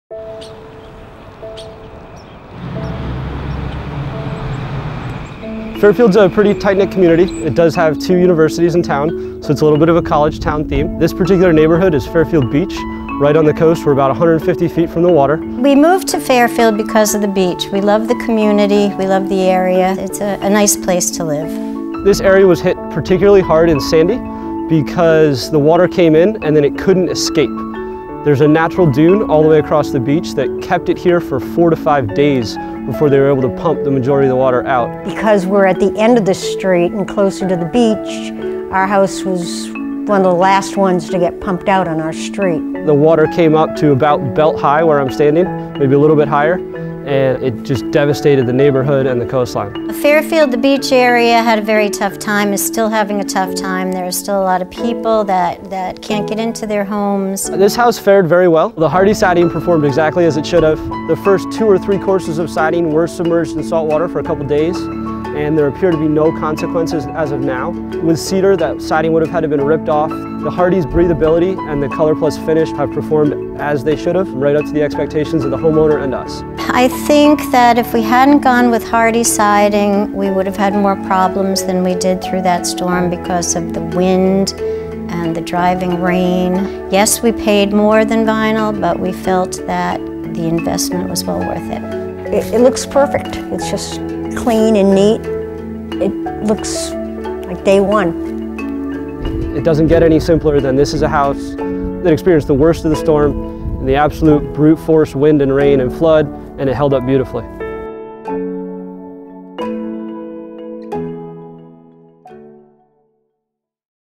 Protecting Homes From Super Storm Sandy with Moisture Resistant Siding
Discover how a home with moisture resistant siding fared in the town of Fairfield, CT, after it was left under four to five feet of water due to Super Storm Sandy. These homeowners' credit James Hardie siding, which is Engineered for Climate, with helping protect their home. Find out all of the other elements James Hardie siding can withstand